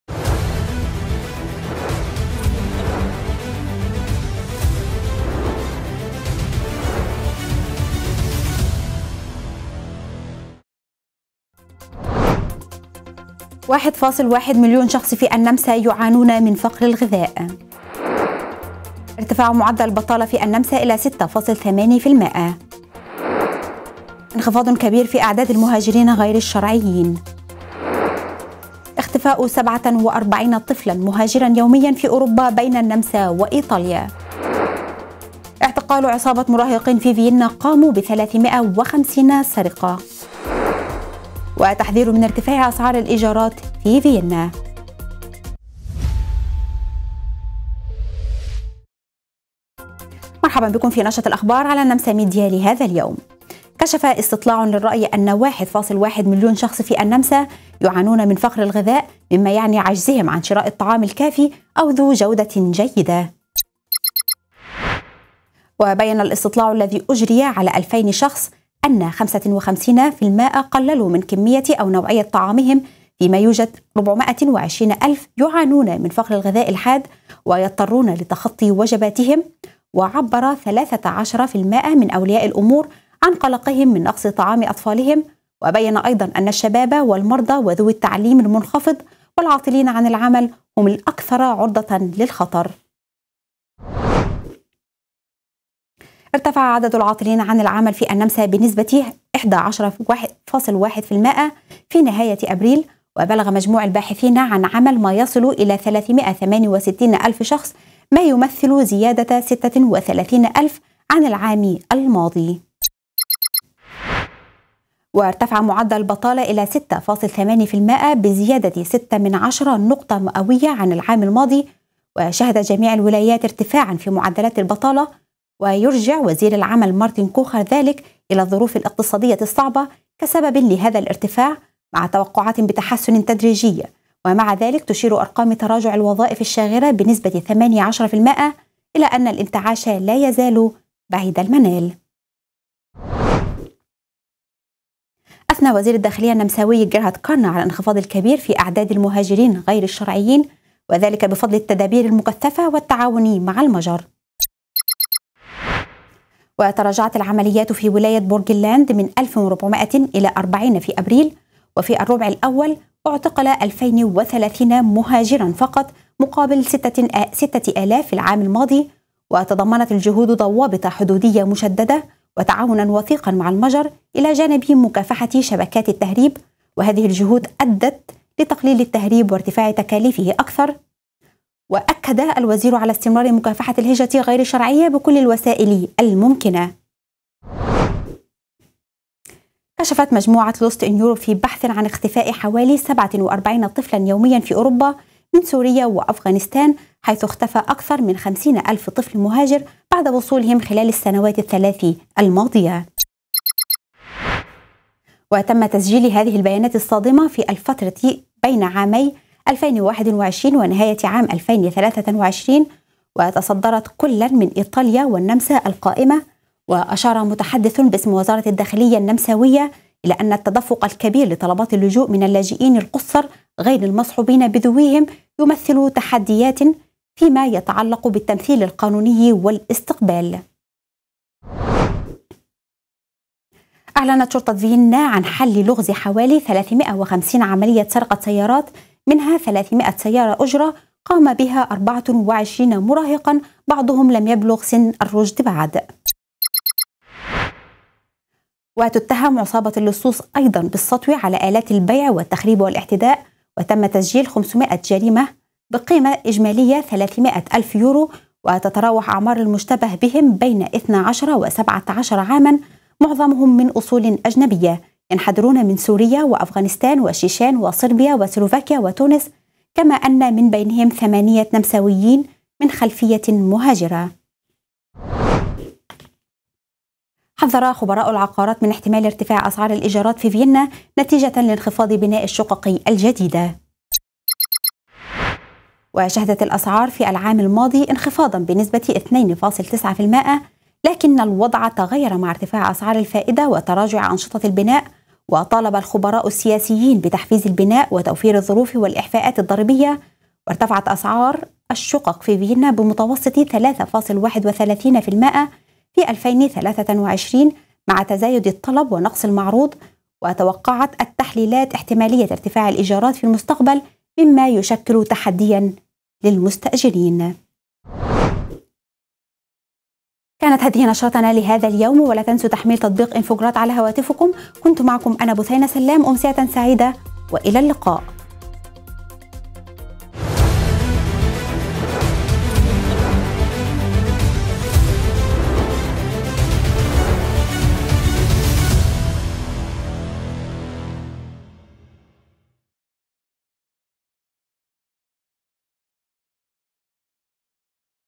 1.1 مليون شخص في النمسا يعانون من فقر الغذاء وإختفاء 47 طفل مهاجر يومياً في أوروبا
1.1 مليون شخص في النمسا يعانون من فقر الغذاء وإختفاء 47 طفل مهاجر يومياً في أوروبا